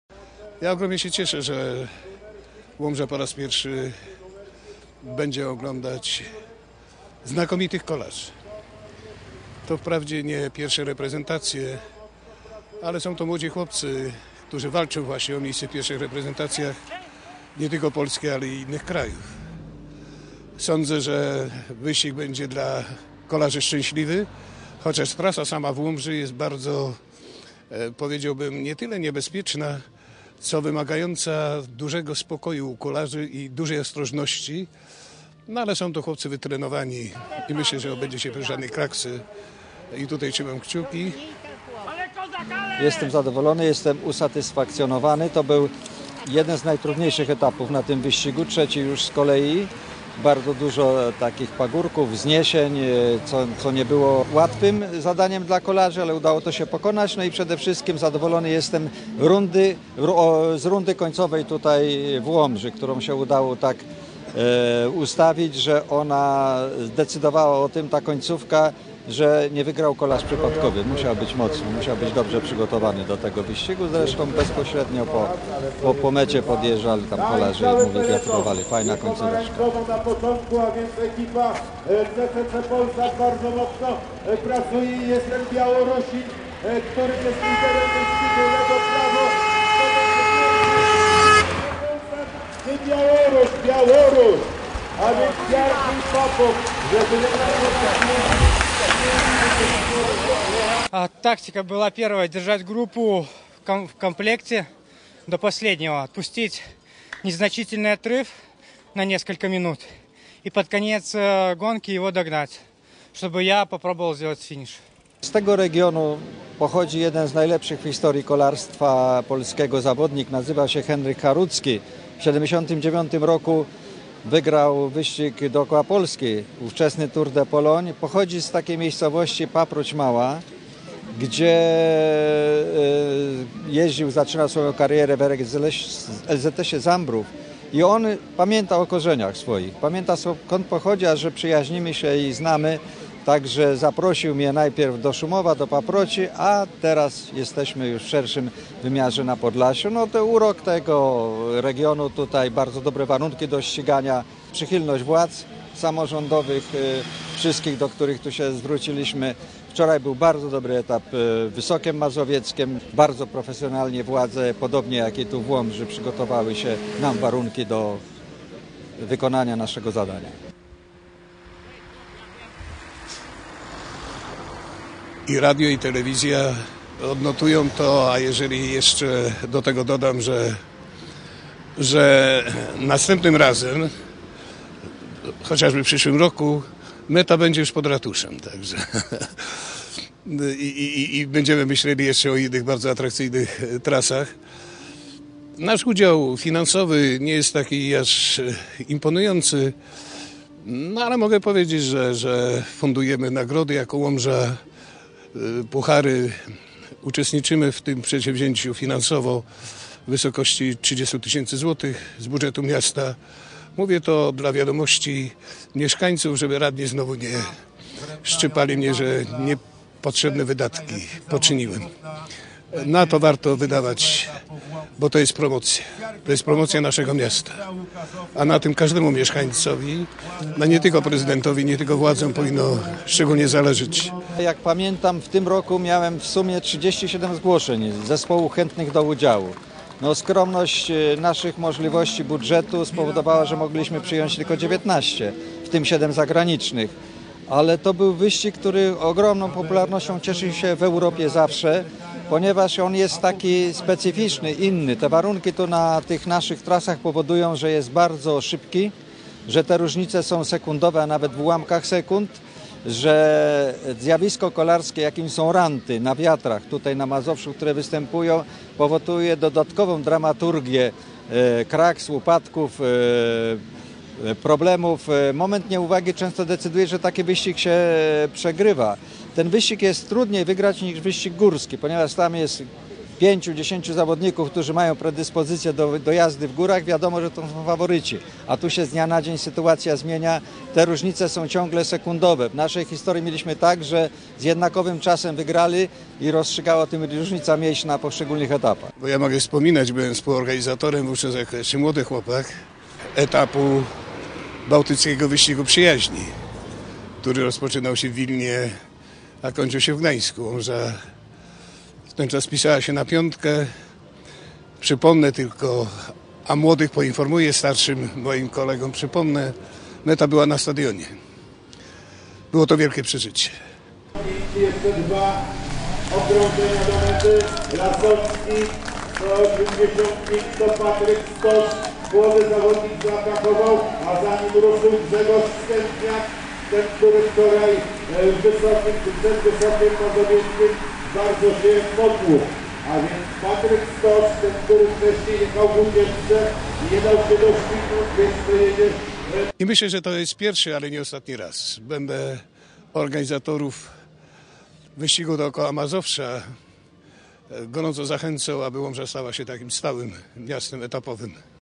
Kolarze ścigali się w Łomży
Trzeci etap 56. Międzynarodowego Wyścigu Kolarskiego Dookoła Mazowsza „Mazovia Tour" miał swój finisz w Łomży.